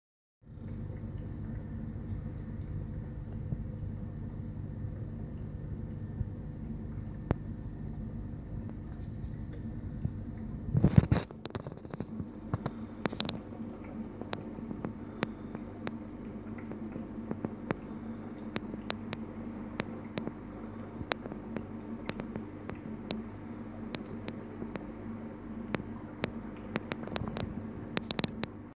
Julidochromis marcsmithi mit Nachwuchs
Die Marcsmithi haben Nachwuchs im Gesellschafts Becken